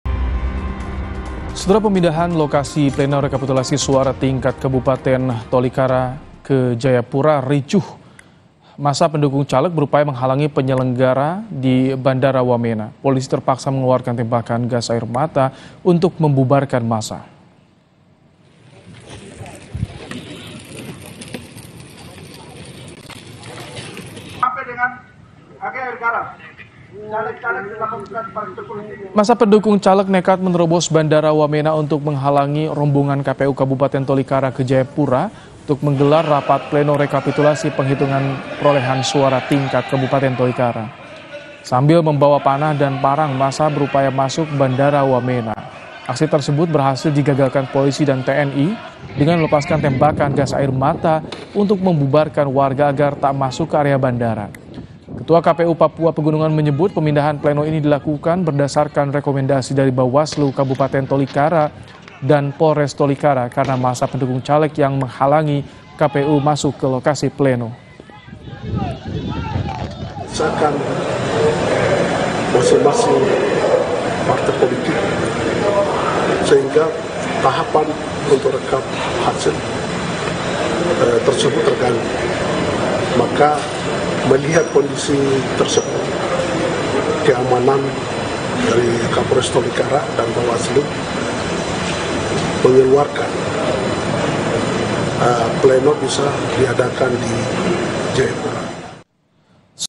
Protes Pemindahan Lokasi Rekapitulasi Oleh KPU Tolikara Papua Ricuh, Massa Sampai Ke bandara Wamena
PAPUA, KOMPAS.TV - Protes Pemindahan Lokasi Rekapitulasi Oleh KPU Tolikara Papua Ricuh, Massa Sampai Ke bandara Wamena